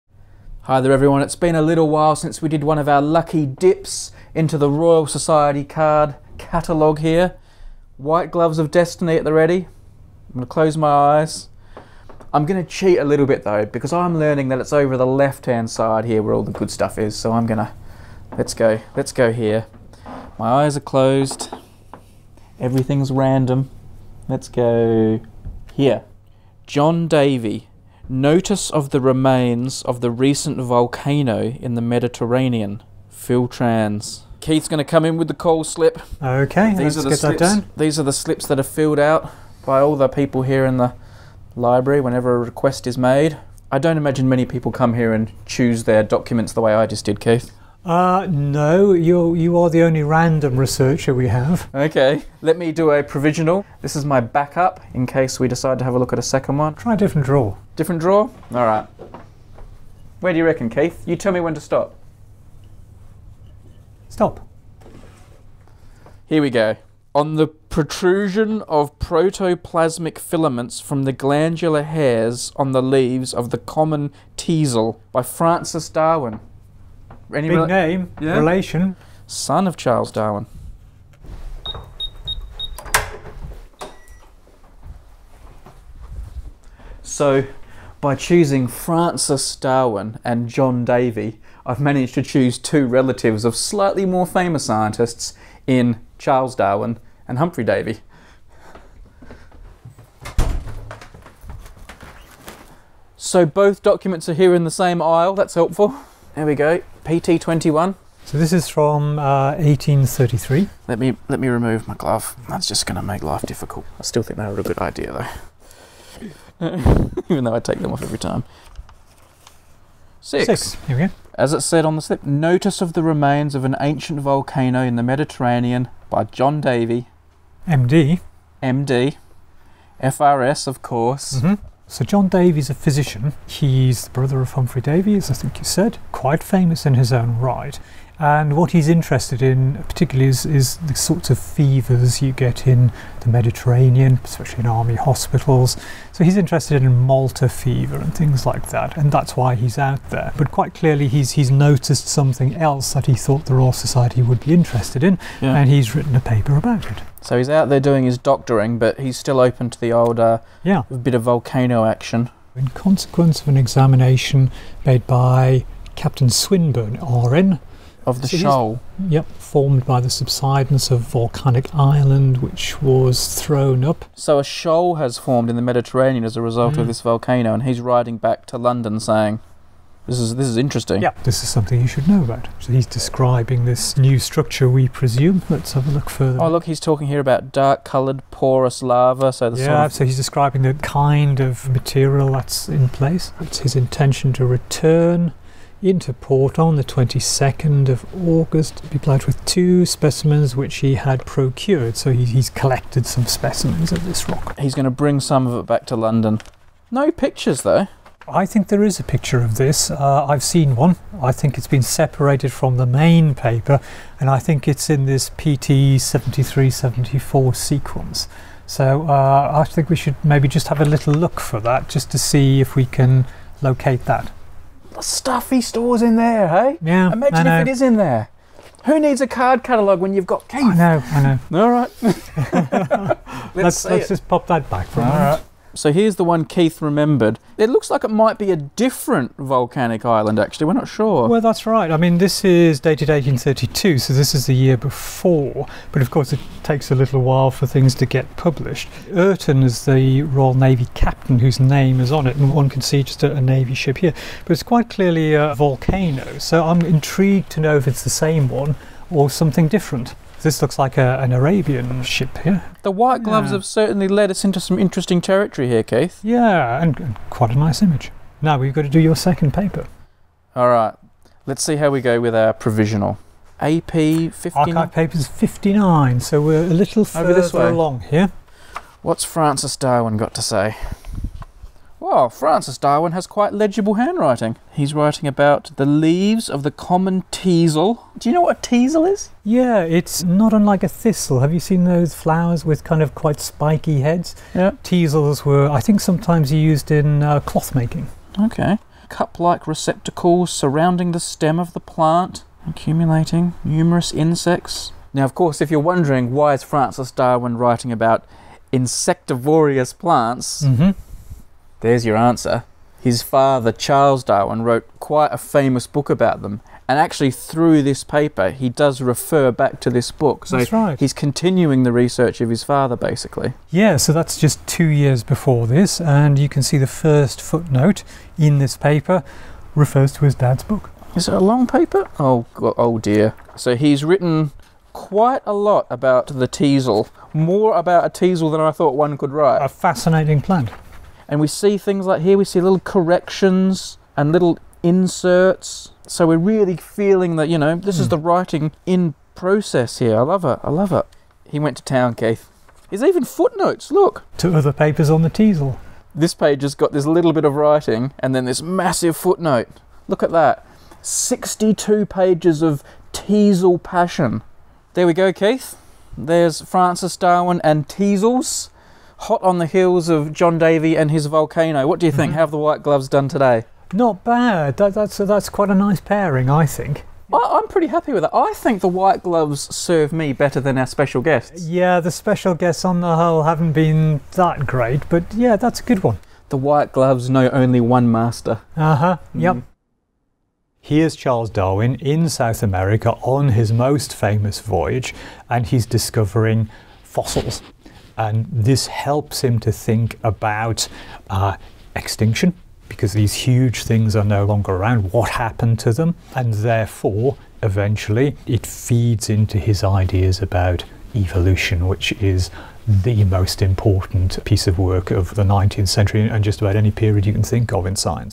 White Gloves of Destiny (Random Researcher) - Objectivity 72
It's another White Gloves of Destiny in which Brady takes a lucky dip into the card catalogue at The Royal Society. What will he find this time?!